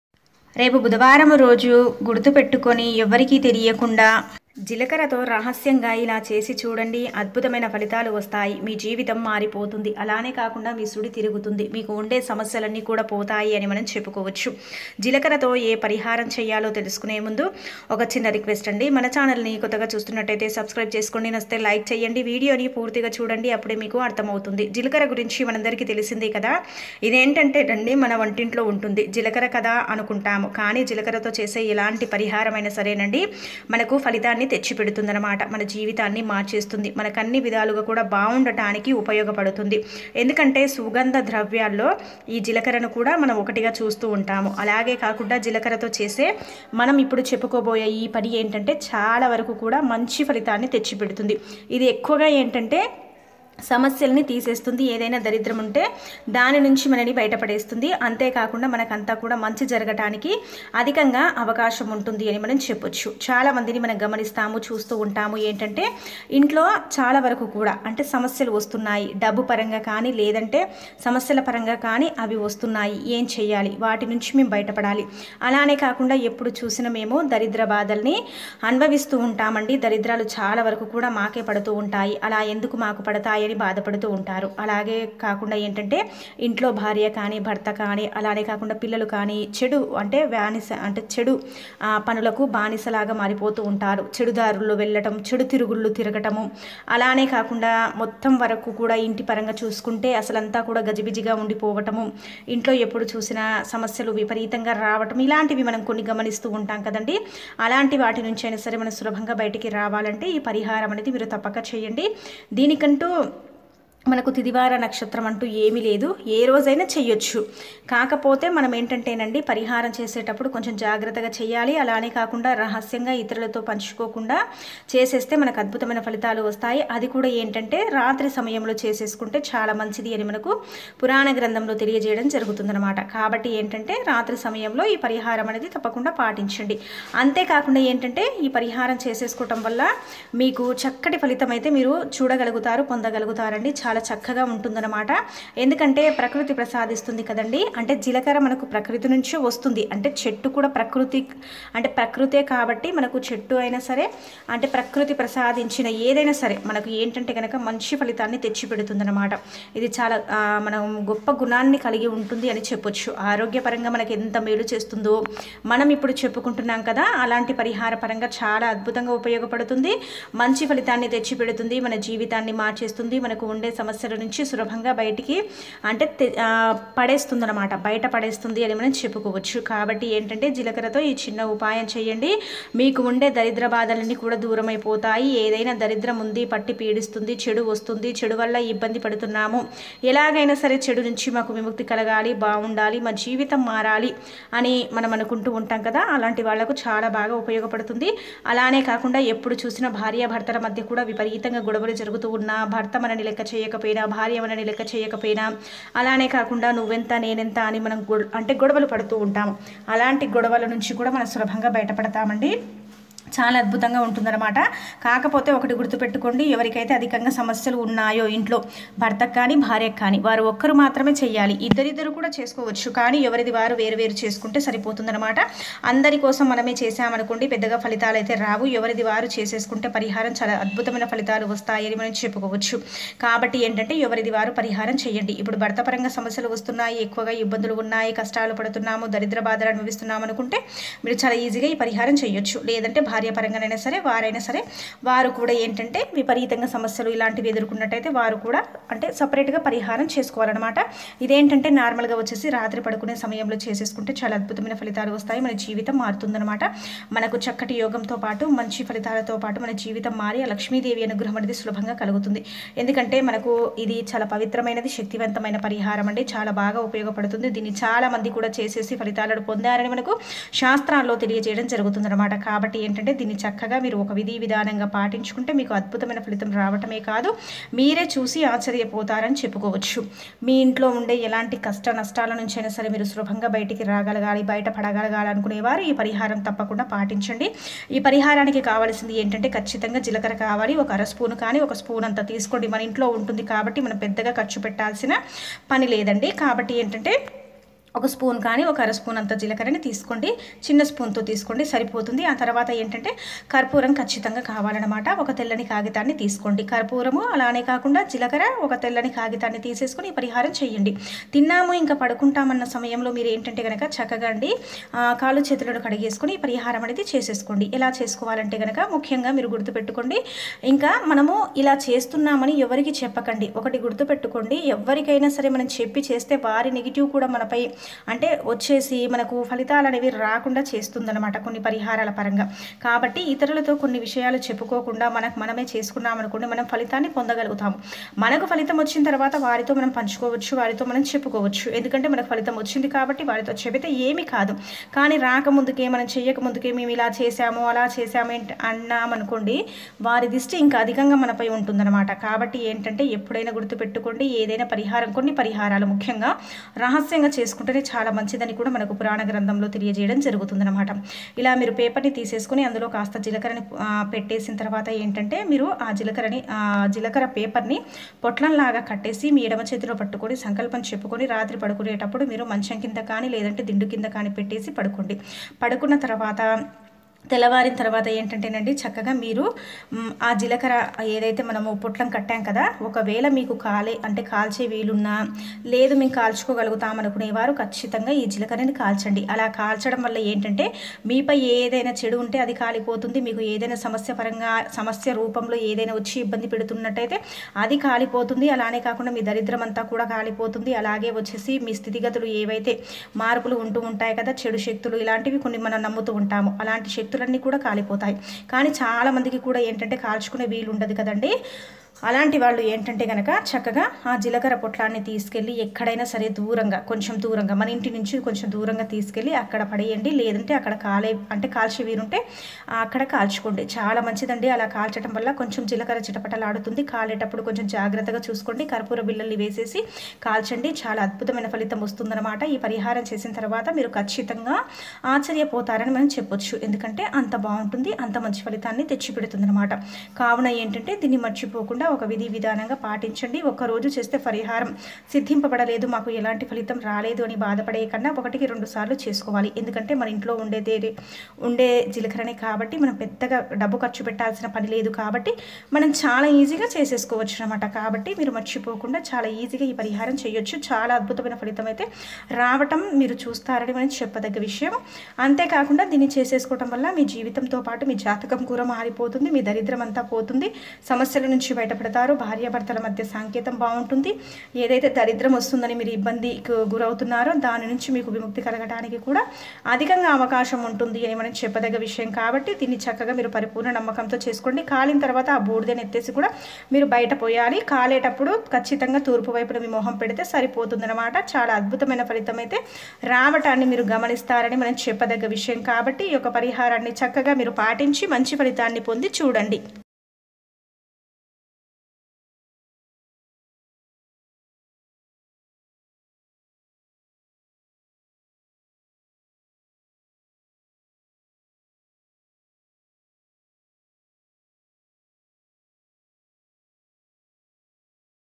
రేపు బుధవారం రోజు రహస్యంగా జీలకర్రతో ఇలా చేస్తే మీ సమస్యలన్నీ పోతాయి మీ సుడి తిరిగి కోటీశ్వరులు..
రేపు బుధవారం రోజు రహస్యంగా జీలకర్రతో ఇలా చేస్తే మీ సమస్యలన్నీ పోతాయి మీ సుడి తిరిగి కోటీశ్వరులు అవుతారు